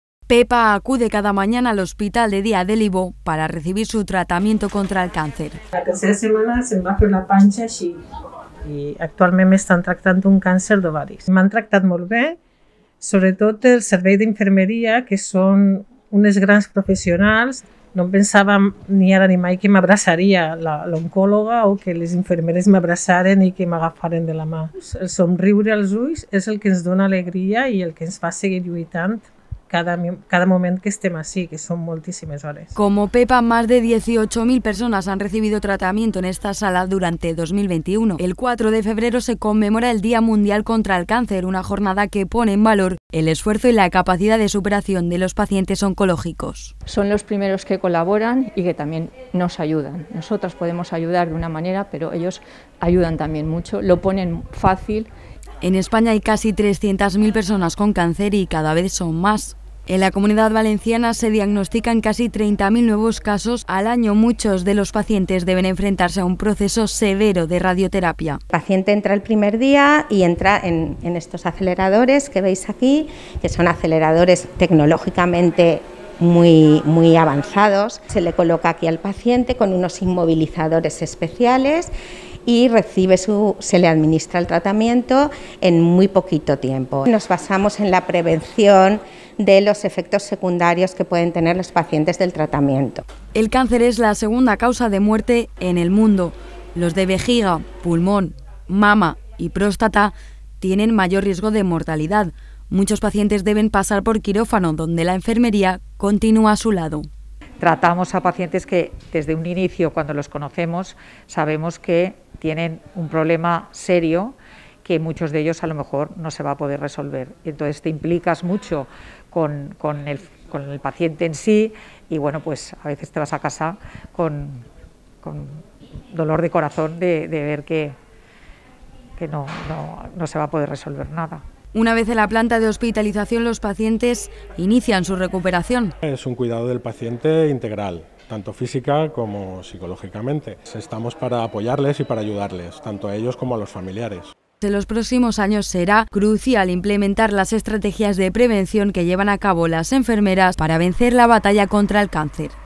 Un día en la piel de las enfemeras oncológicas del IVO | Cecova TV
Pepa acude cada mñana al hospital de día del Instituto Valenciano de Oncología (IVO) para recibir su tratamiento contra el cáncer. Como Pepa, más de 18 mil personas han recibido tratamiento en este hospital durante 2021. El 4 de febrero se conmemora el Día Mundial contra el Cáncer. Una jornada que pone en valor el esfuerzo y la capacidad de superación de los pacientes oncológicos.